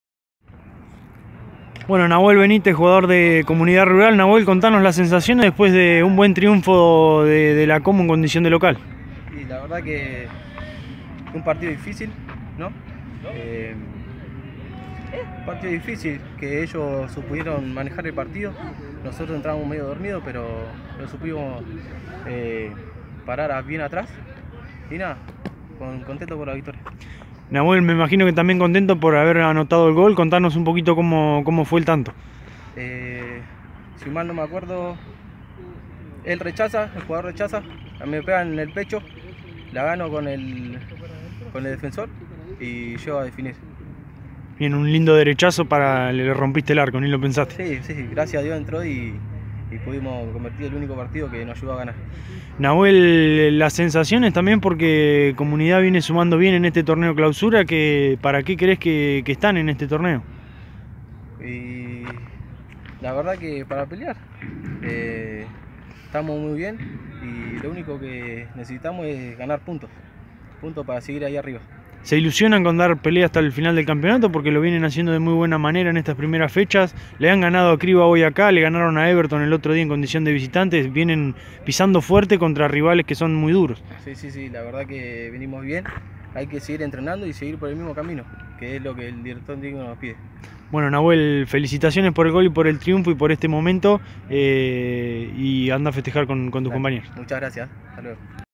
L.A.P. CL. '19 - SEXTA FECHA. COMUNIDAD RURAL 1 - 0 CRIBA
La palabra de Nahuel Benítez, autor del tanto en Los Hornos para la Comu.